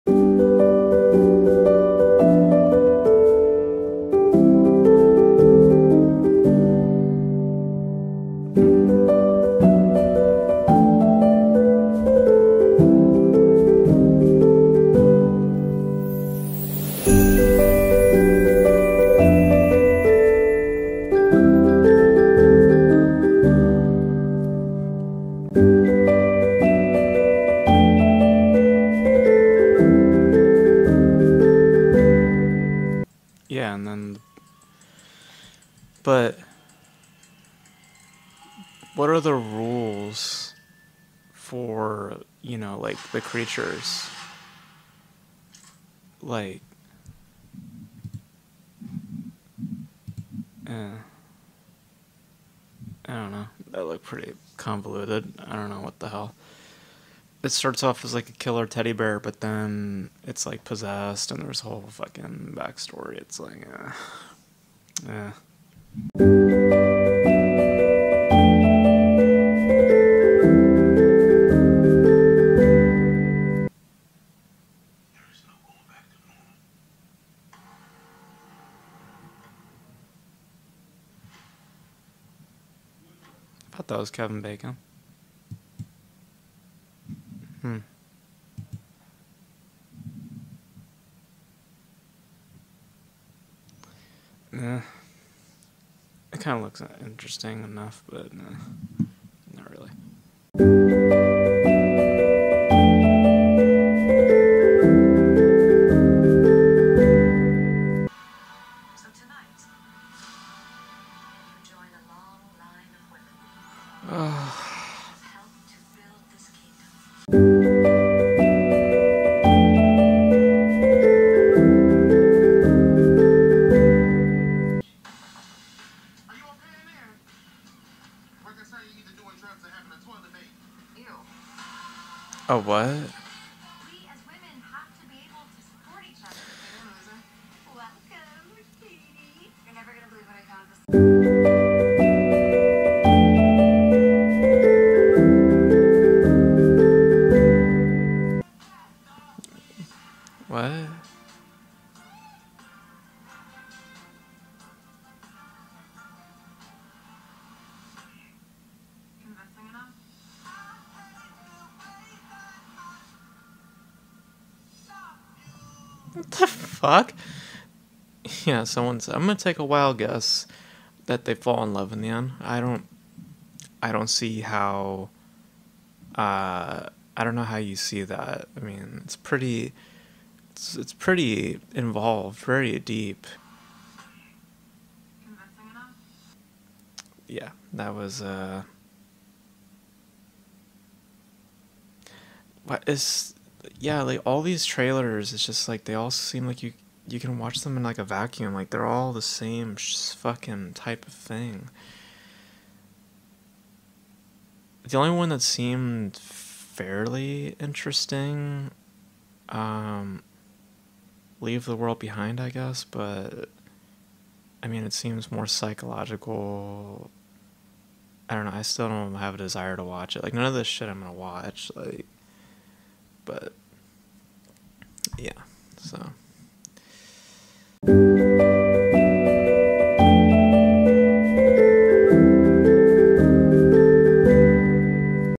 My Trailer 'Reactions'
Here's my "distinct" reactions to the recent trailers that have come out